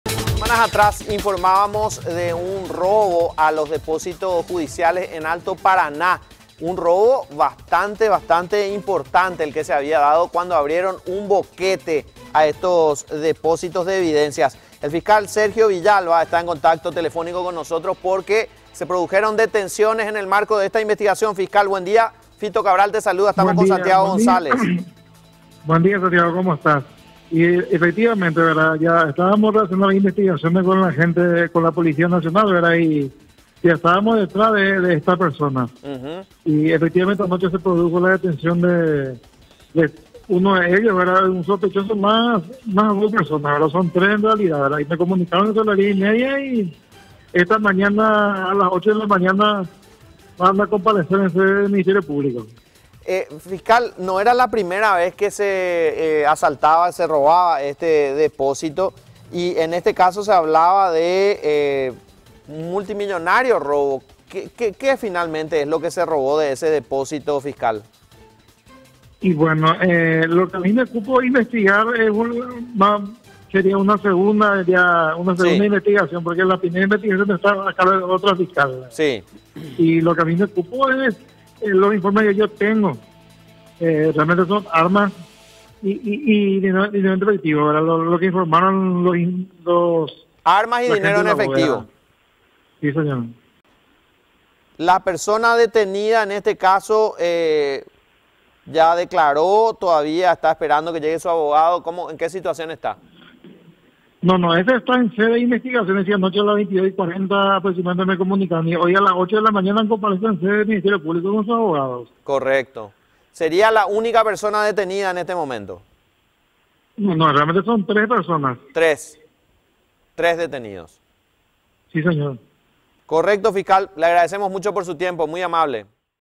Tres detenidos por robo en la bóveda del Poder Judicial de CDE
La Policía realizó un operativo y logró detener a tres hombres que serían los que robaron en la bóveda del Palacio de Justicia de Ciudad del Este. Del lugar se extrajeron armas, dinero en efectivo y otros elementos.